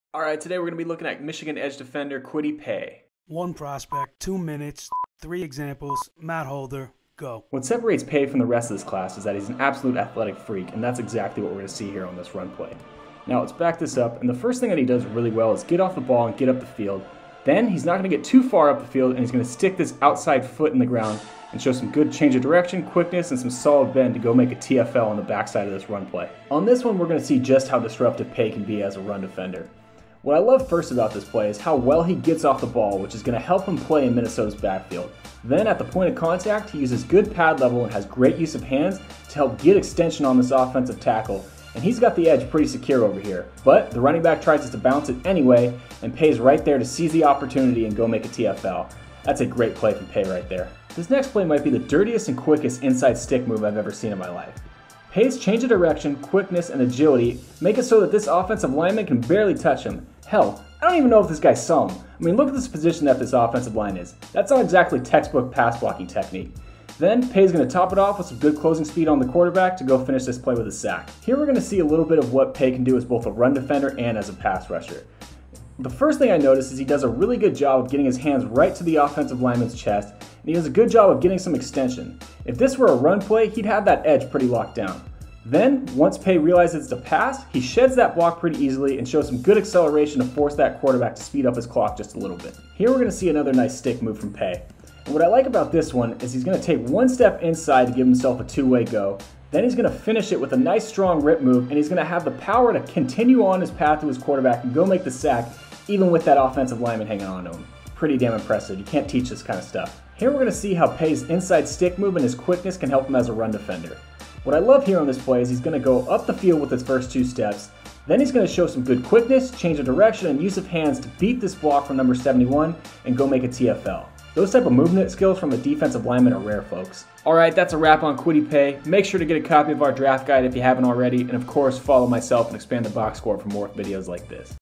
Kwity Paye 220 Scouting | Expand The Boxscore
Kwity Paye is downright scary. The #10 overall player on the XTB Big Board, and top EDGE in this year's NFLDraft class. @MHolder95 walks you thru what makes him so dominant and why he should be a priority for teams picking at the top (and IDP leagues).

Superior Stats. Dominate Football. ExpandTheBoxscore.com